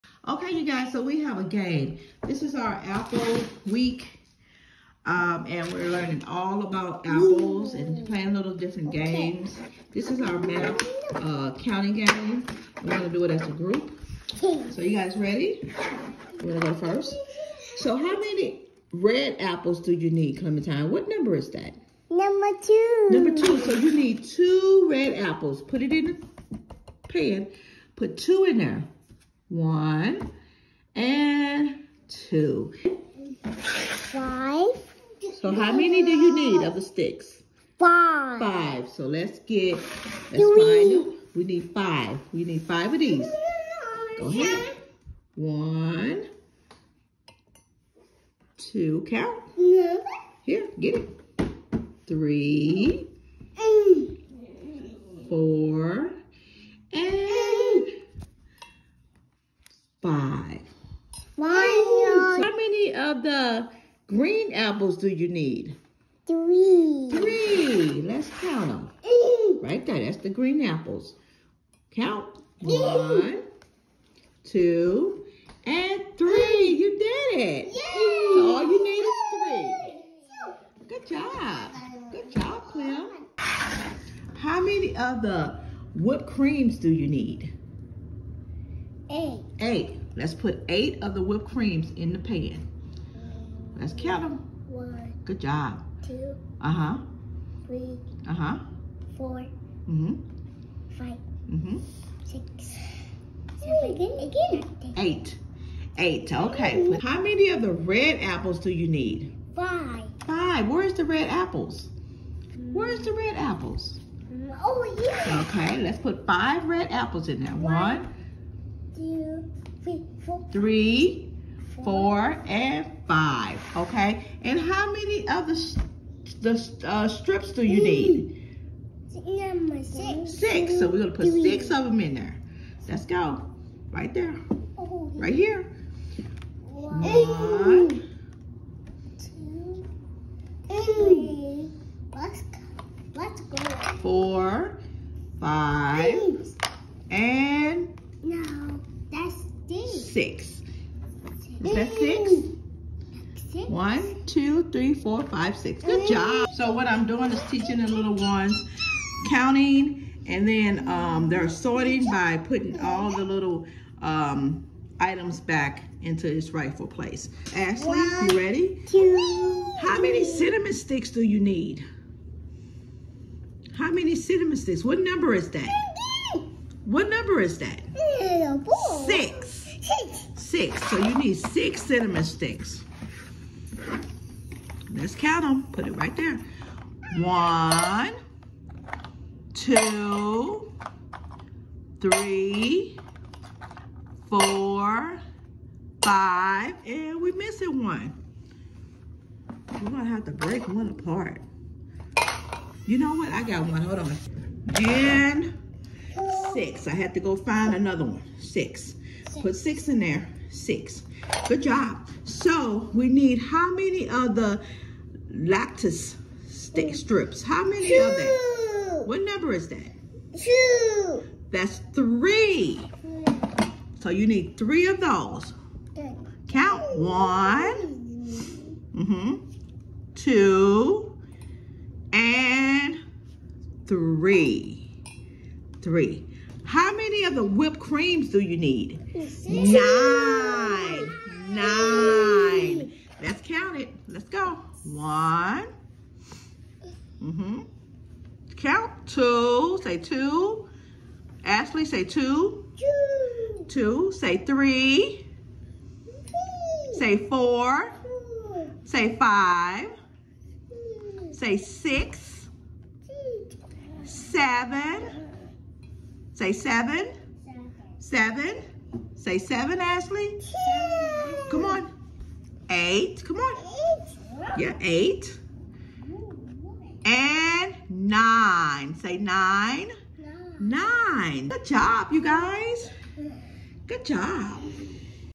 Day in the life of a small home learning academy * How to teach counting-sorting to little learners
Thanks for being our friends ❤️
Continue to spread Love ❤️